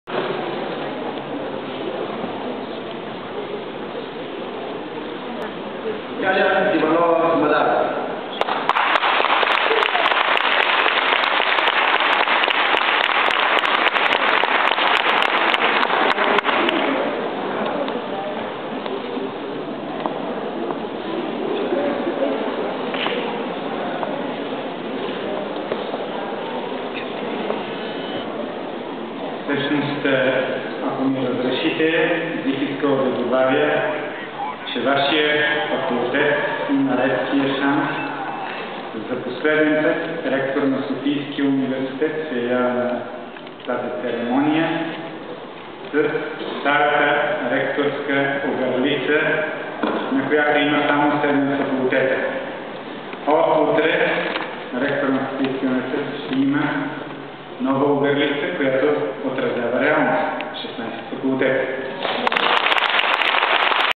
19.11.2008 - Sofia University - Realnostite - 16 fakulteta
Ректорът на Софийския Университет за последен път присъства на церемония със старата си ректорска огърлица, на която пише 7 факултета. Вече е с новата, която отразява реалностите - 16 факултета.